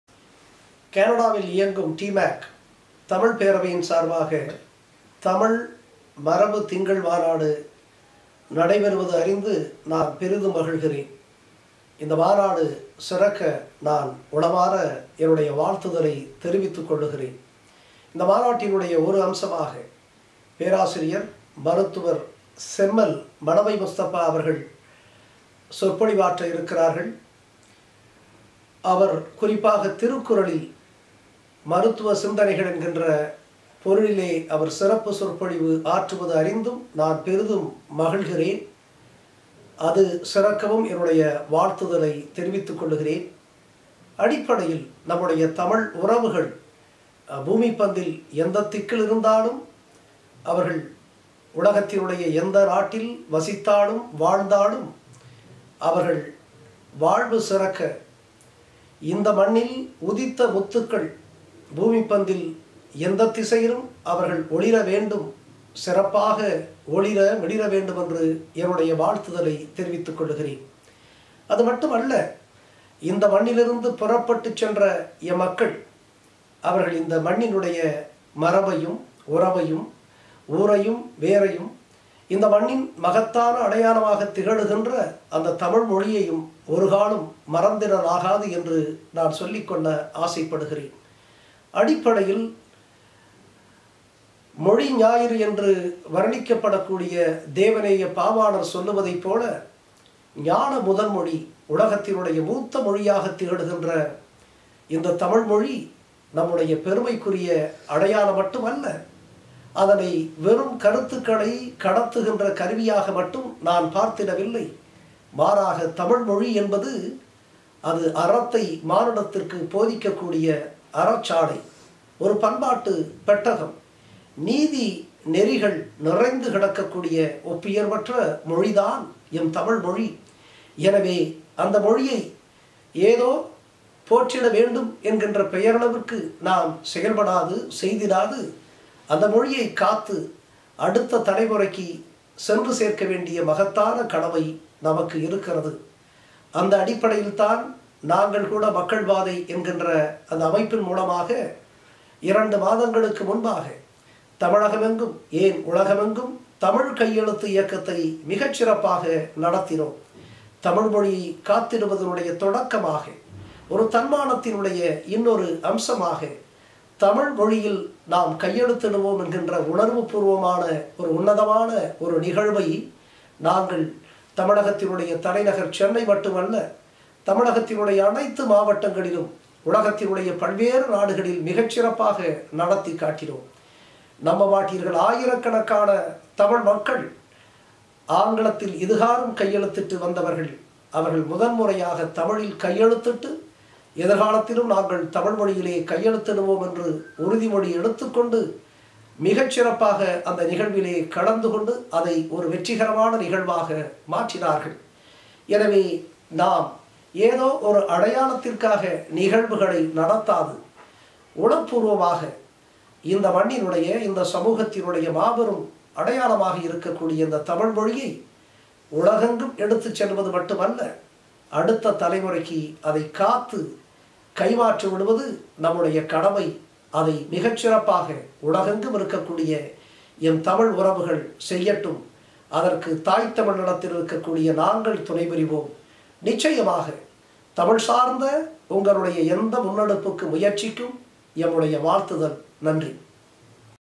கனடாவாழ் தமிழர்களுக்கு உயர்திரு சகாயம் IAS அவர்களின் வாழ்த்துச் செய்தி
கனடாவாழ் தமிழர்களுக்கு
உயர்திரு சகாயம் IAS
அவர்களின்
வாழ்த்துச் செய்தி